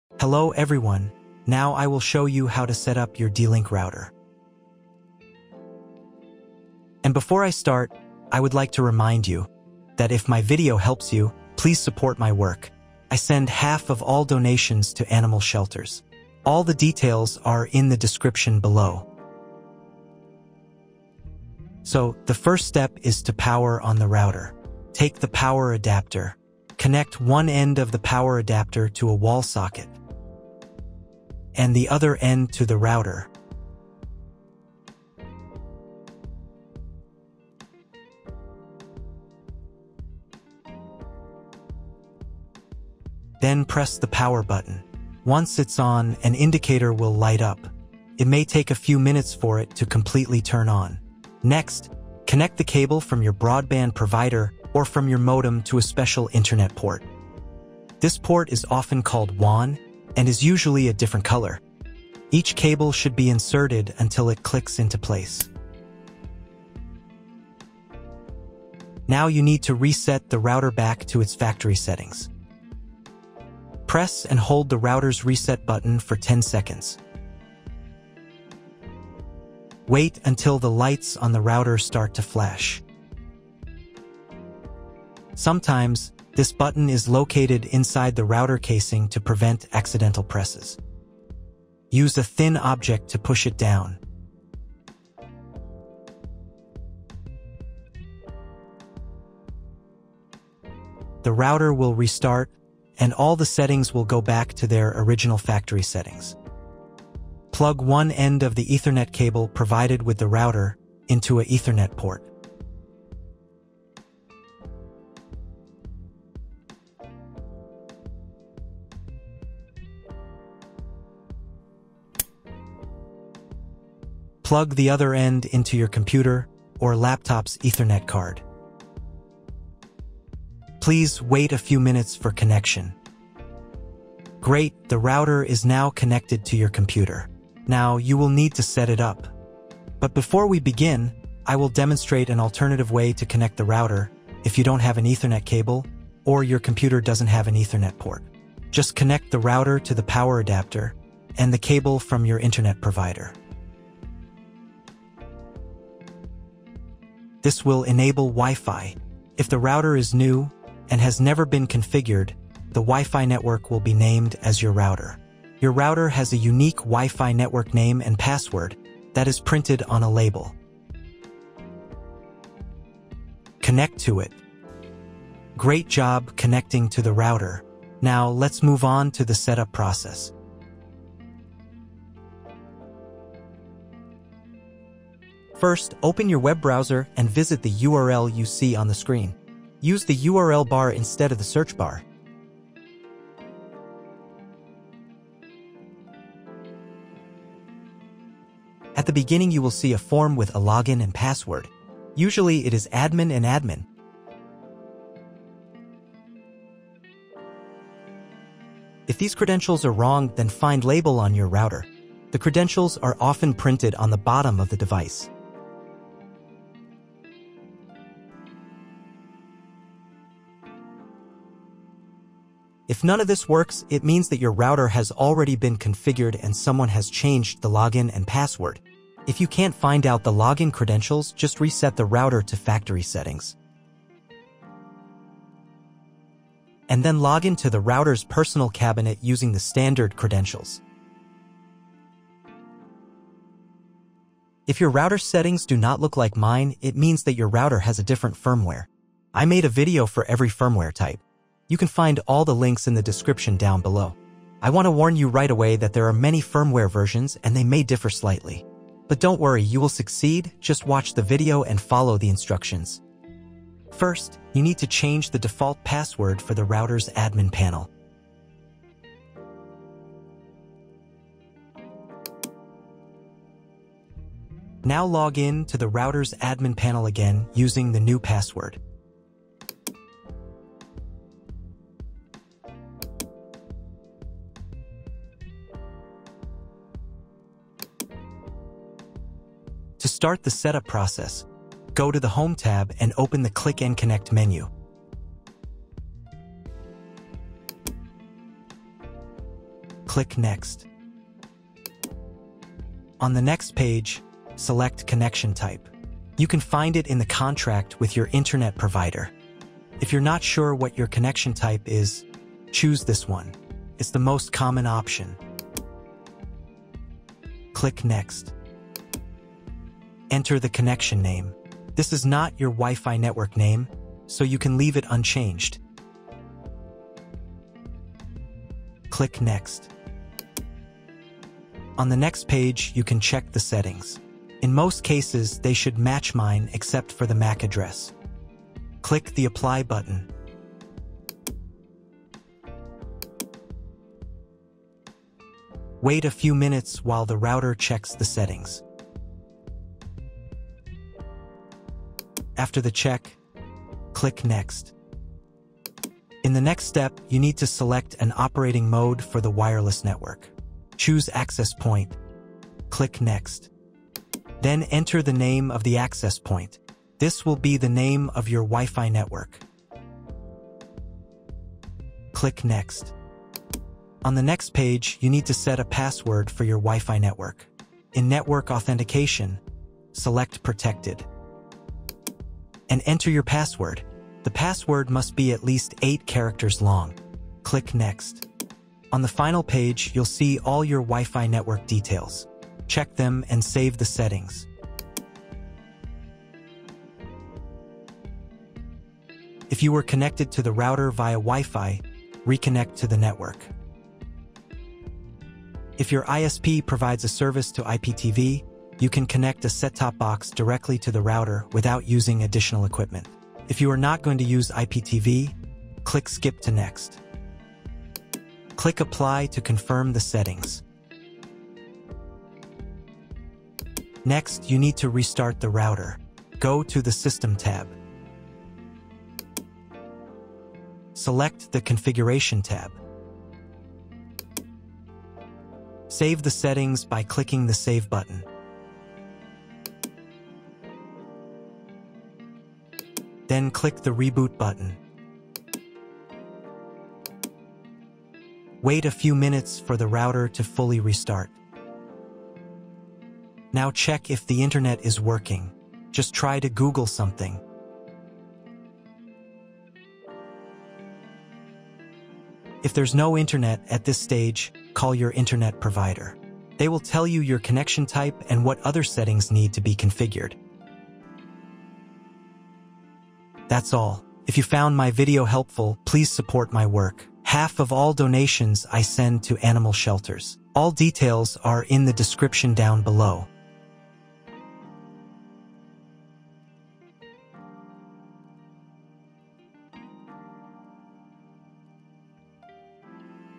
✅ How to Set Up D-Link DIR-615 WiFi Router | How to Setup D-LINK N300 DIR-615
In this video, I will show you how to set up D-Link DIR-615. First, I will show you in detail how to connect computer to router with ethernet cable. Then  how to login into your routers setting. How to set Wifi Password, how to change WiFi name and password and how to reset a router.

🟡BTC: bc1q6lxzusxt27nl25fwcluw7g72uveat2gr7q4as7
🟡ETH: 0xA93CabcE97Ec2c20fc08382283a2CC95d6579EFB
🟡USDT (TRC20): TANkFPkipQ66cFacAe85PiZSq6cEJfBBRQ
🟡USDT (ERC20): 0xA93CabcE97Ec2c20fc08382283a2CC95d6579EFB

Chapters:
00:00 – Intro
00:20 – Connect the router DIR-615
01:19 – Reset settings on the router DIR-615
03:13 - Setup the router DIR-615